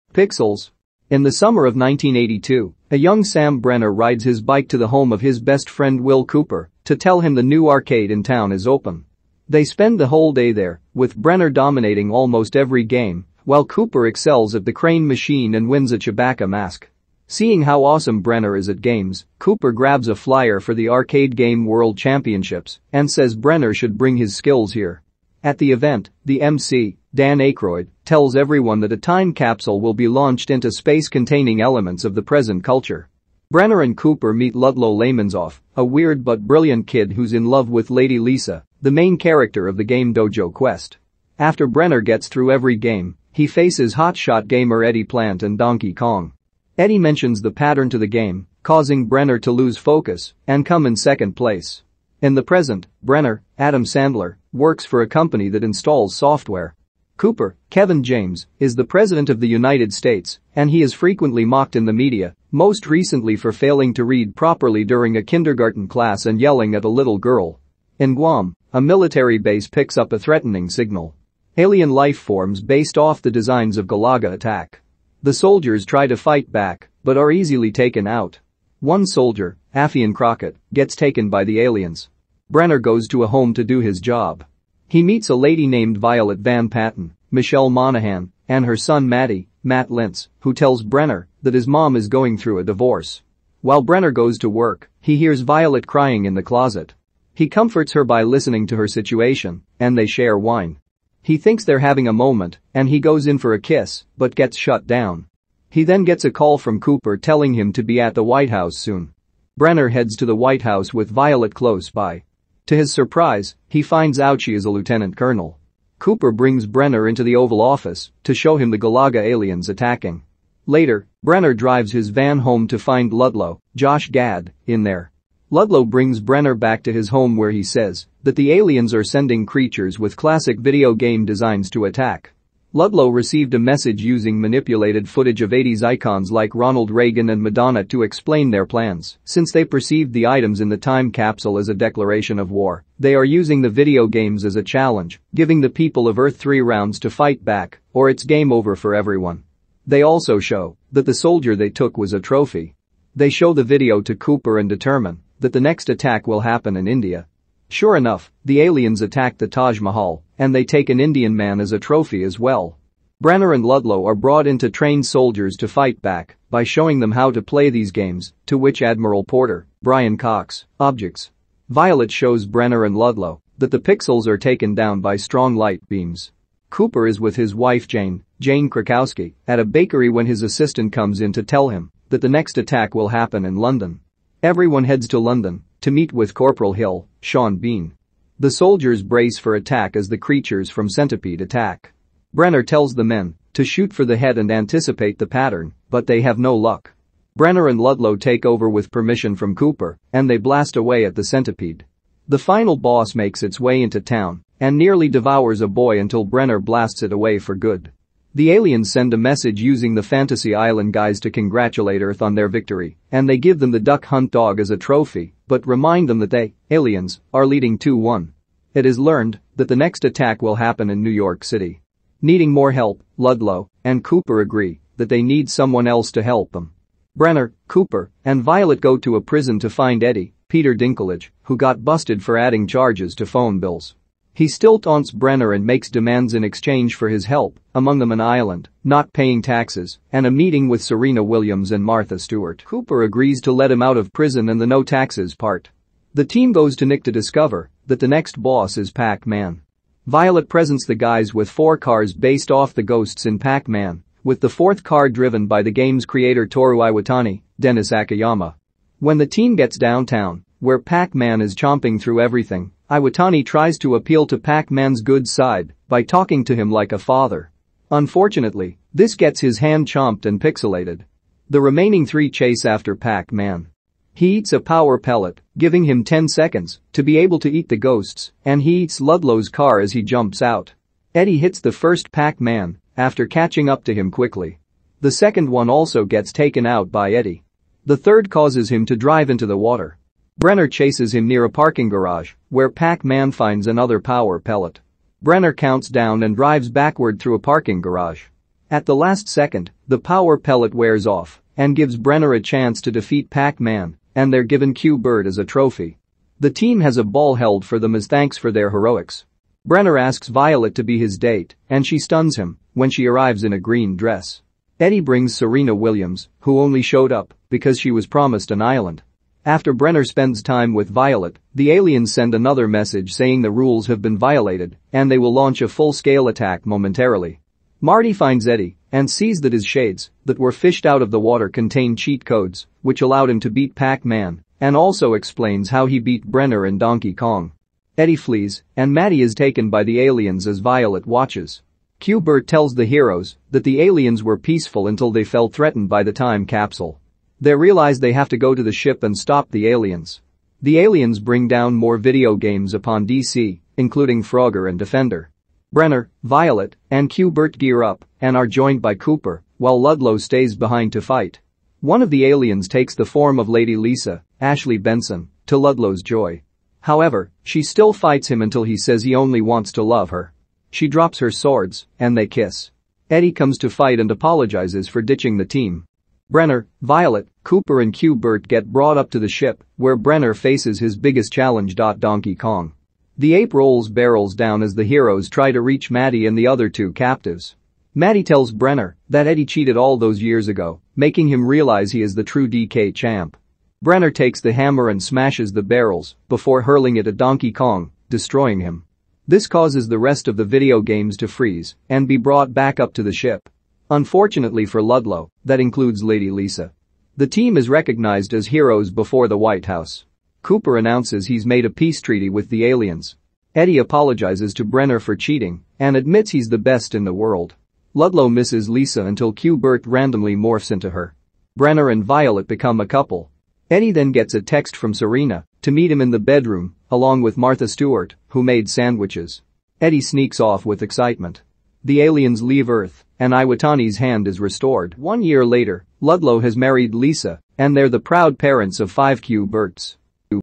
[REVIEW] PIXELS
PIXELS
Release: 2015
Time: 1h 45m
Genres: Action, Comedy, Science Fiction
Language: English
Rating: 5.7/10
Countries: China, United States of America
Production: Columbia Pictures, Happy Madison Productions, 1492 Pictures, LStar Capital, China Film Group Corporation, Sony Pictures
Casts: Adam Sandler, Kevin James, Michelle Monaghan, Peter Dinklage, Josh Gad, Sean Bean, Brian Cox, Ashley Benson, Dan Aykroyd, Jane Krakowski